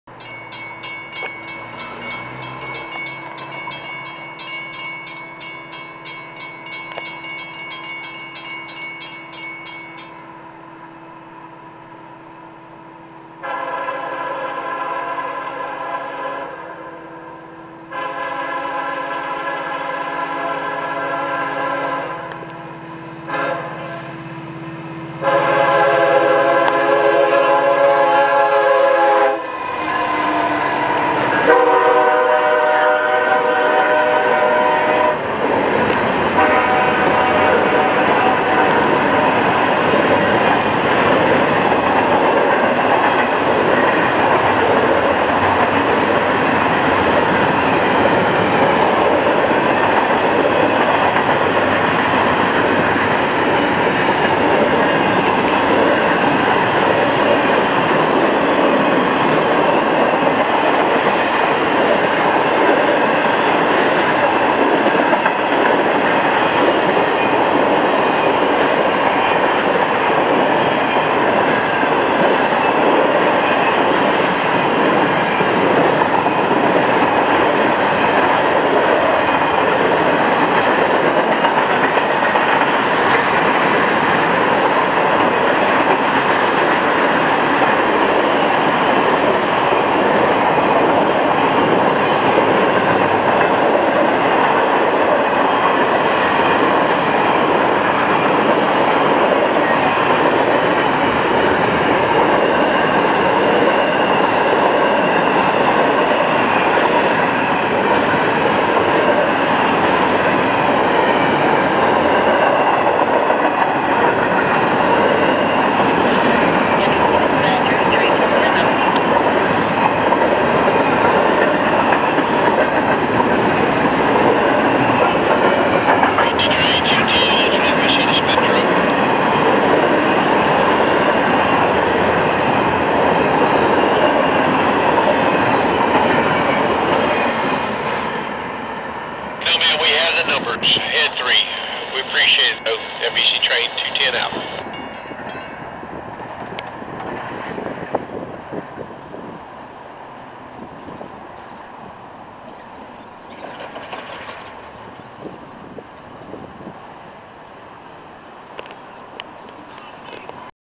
FEC210 Dec 22, 2011
Across Micco Road MP209 with 103 and BigBlue142 - NO Heritage714 - and all IM at 8:26am after swapping in FO.